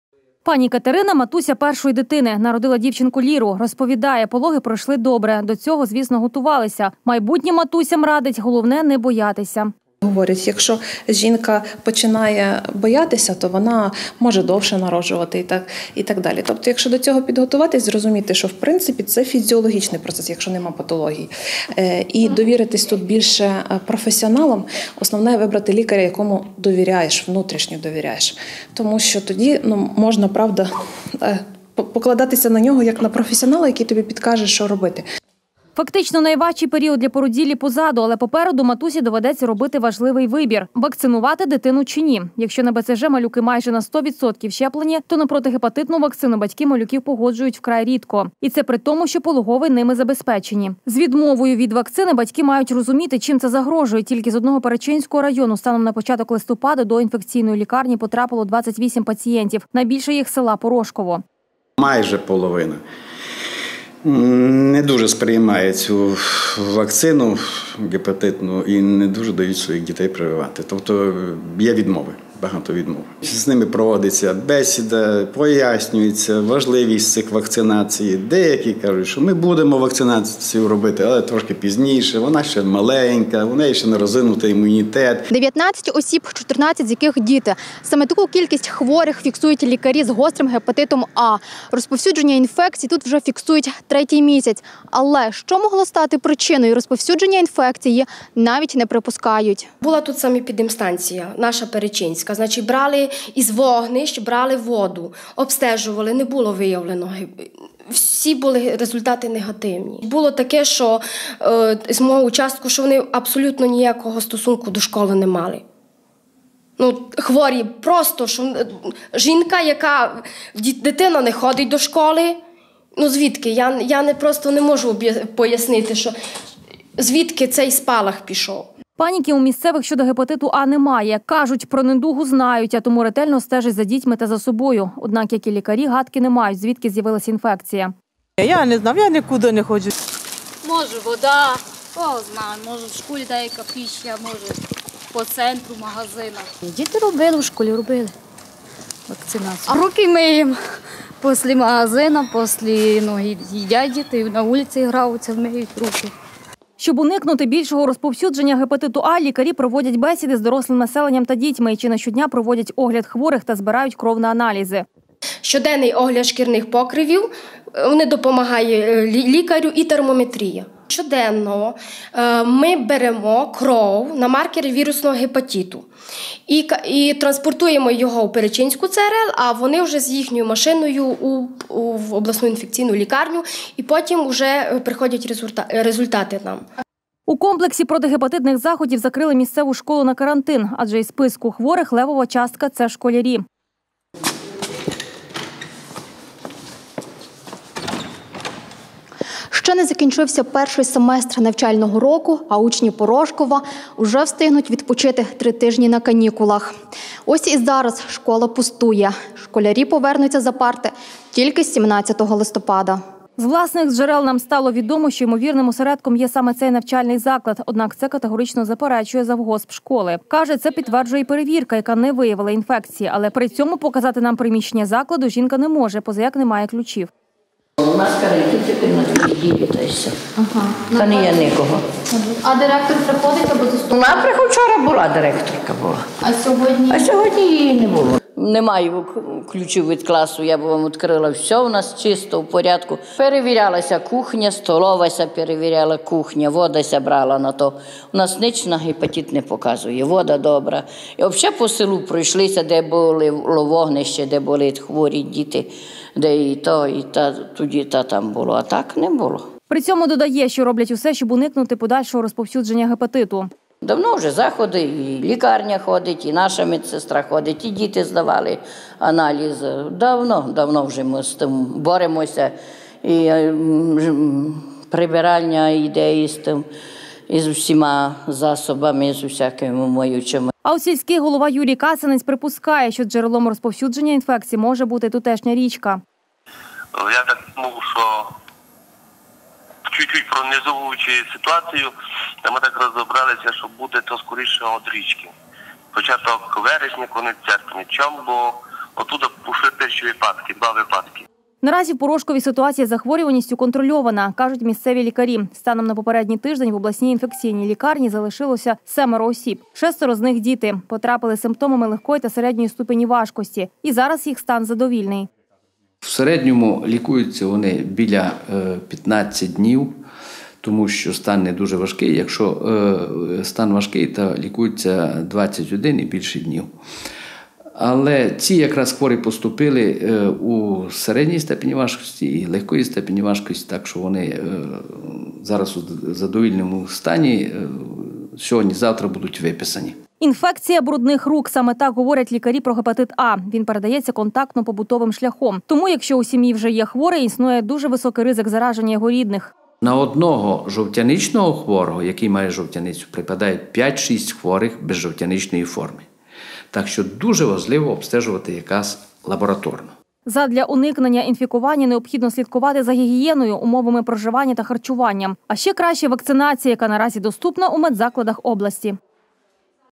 З гепатитом А
Ледь не половина батьків новонароджених відмовляються від протигепатитної вакцини. І це при тому, о на відміну від попереднього року, зараз пологові будинки нею забезпечені. Останнім часом істотно збільшилася кількість хворих з гострим гепатитом А. Нещодавно тільки на Перечинщині було щонайменше три десятки хворих.